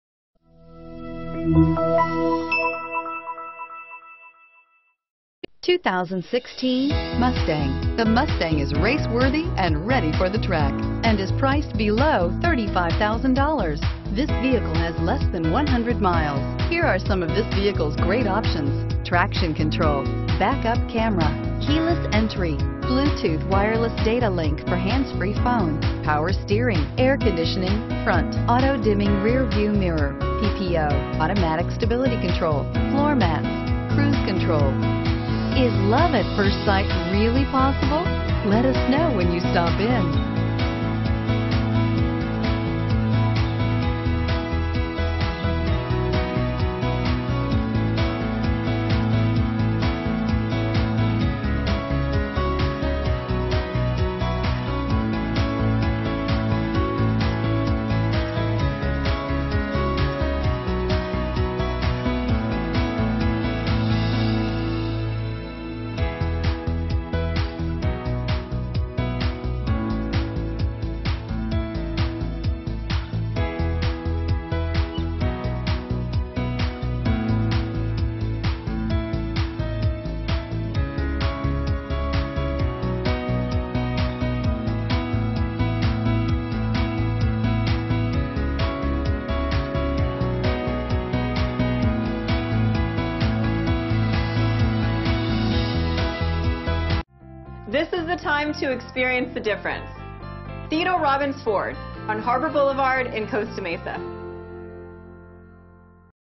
2016 Ford Mustang COSTA MESA,HUNTINGTON BEACH & IRVINE G230807
2016 Ford Mustang 2DR FASTBACK ECOBOOST

THEODORE ROBINS FORD PROUDLY SERVING COSTA MESA, HUNTINGTON BEACH, IRVINE, NEWPORT BEACH, ORANGE COUNTY, AND SURROUNDING SOUTHERN CALIFORNIA LOCATIONS. THIS BLUE 2016 Ford MUSTANG IS EQUIPPED WITH A EcoBoost 2.3L I4 GTDi DOHC Turbocharged VCT, AUTOMATIC TRANSMISSION, AND RECEIVES AN ESTIMATED 22 City/31 Hwy MPG. 2016 Ford MUSTANG 2DR FASTBACK ECOBOOST Stock #: G230807 | VIN: 1FA6P8TH9G5230807 | 2016 Ford Mustang 2dr Fastback EcoBoost Theodore Robins Ford